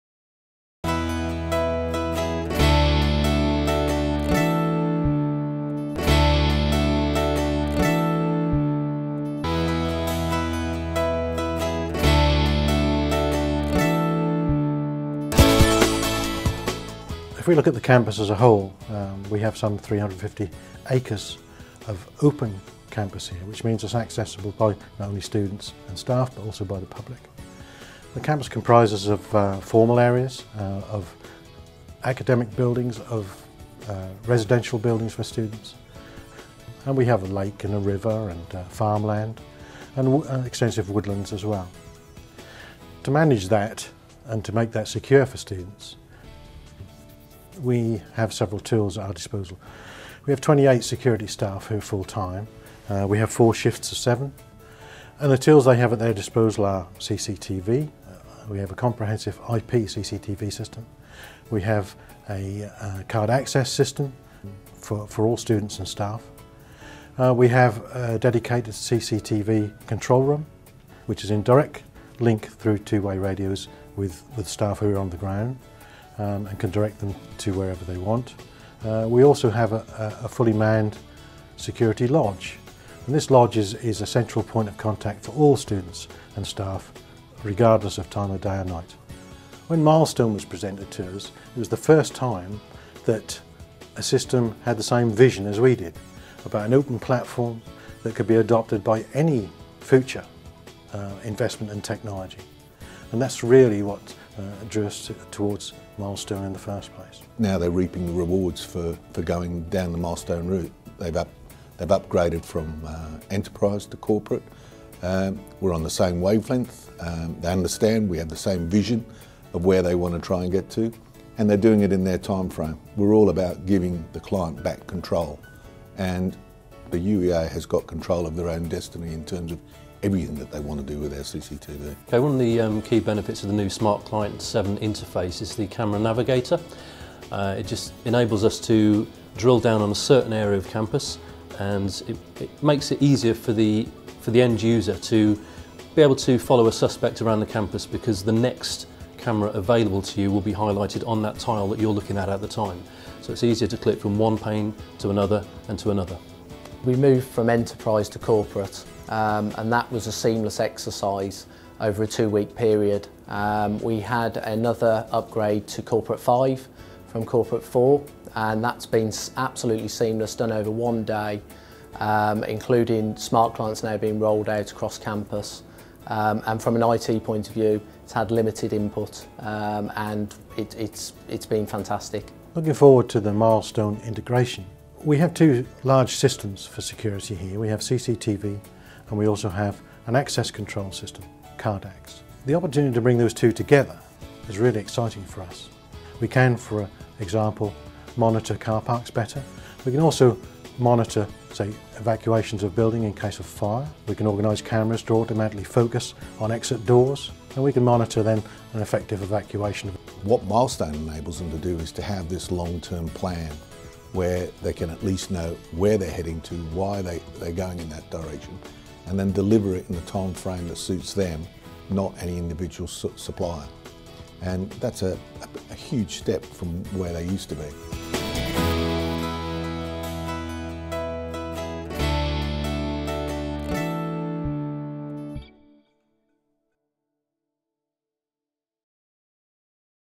University of East Anglia - Milestone Video Management Software Video Case Study
We shot this video for Video Management System market leader Milestone Systems at one of their UK flagship customers - The University of East Anglia. The story focuses on an upgrade from XProtect Enterprise to XProtect Corporate and details plans for full integration with campus-wide Gallagher access control system.